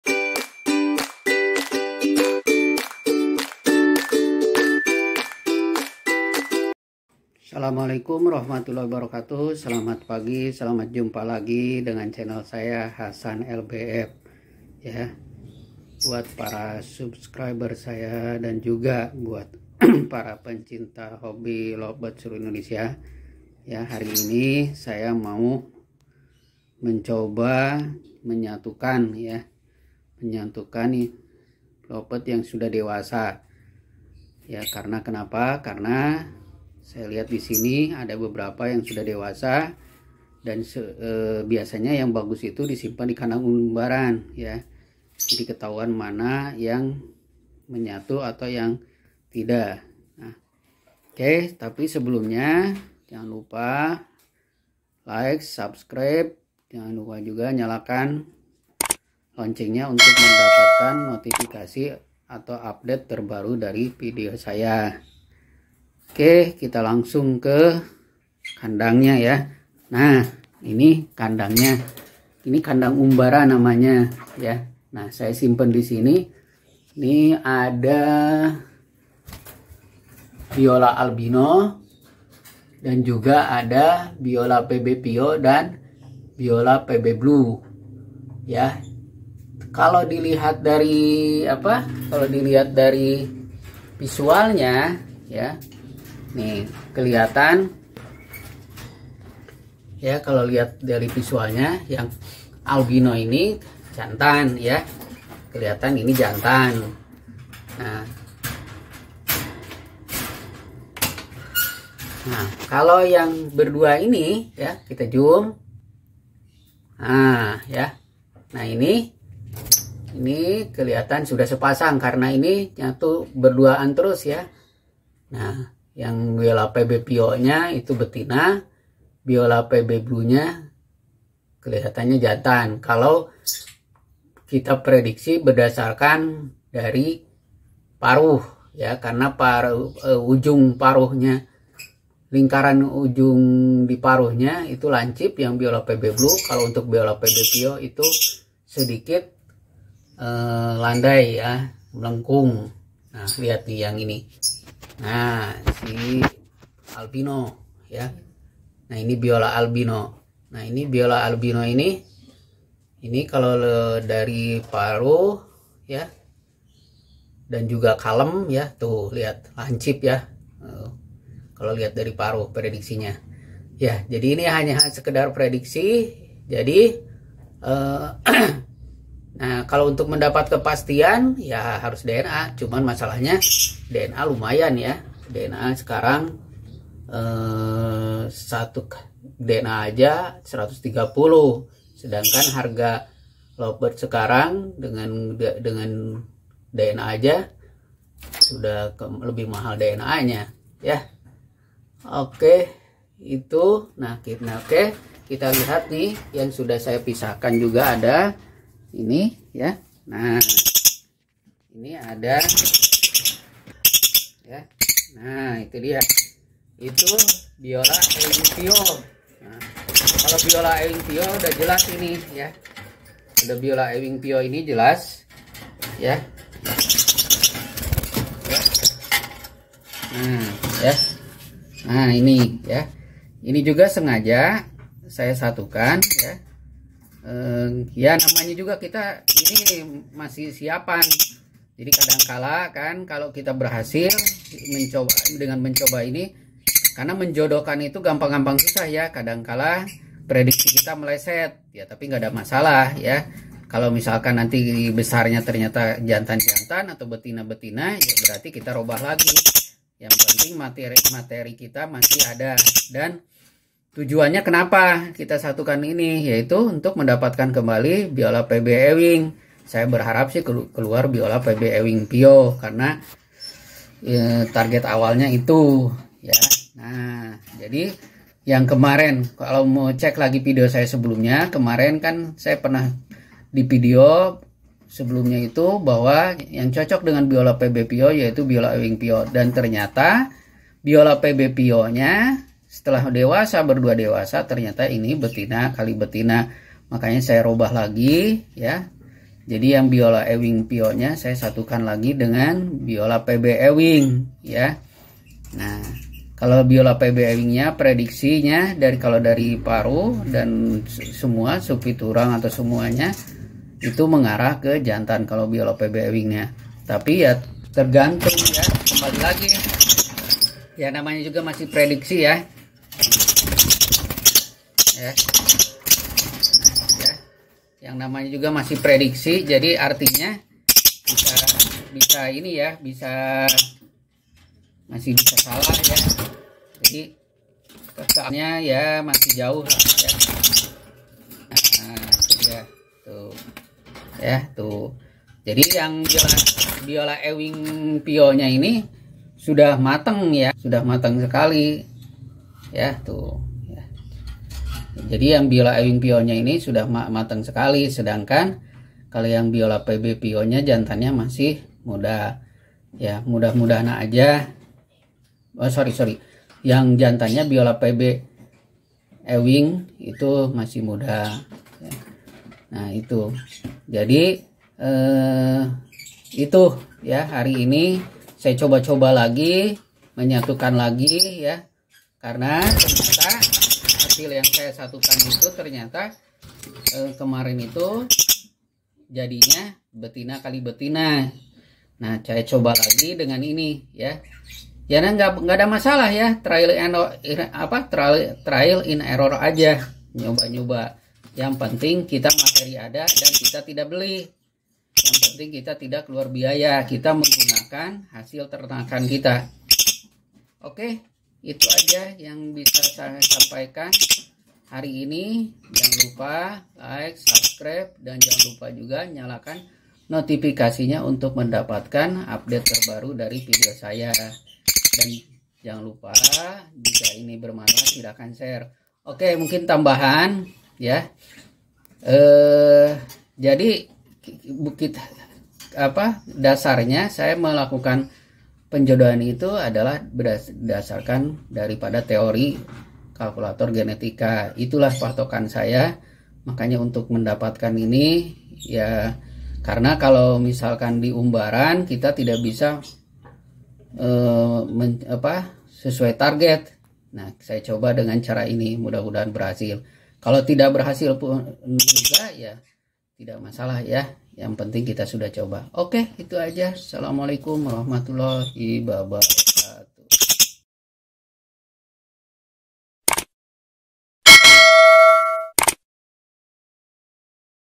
Ternak lovebird bongkar pasangan kembali karena jenis kelamin sama
Pada video saya sebelumnya saya memasangkan biola pb vio dengan biola euwing vio dan ternyata ketahuan jenis kelaminnya sama yaitu betina, oleha karena itu saya bongkar pasang kembali.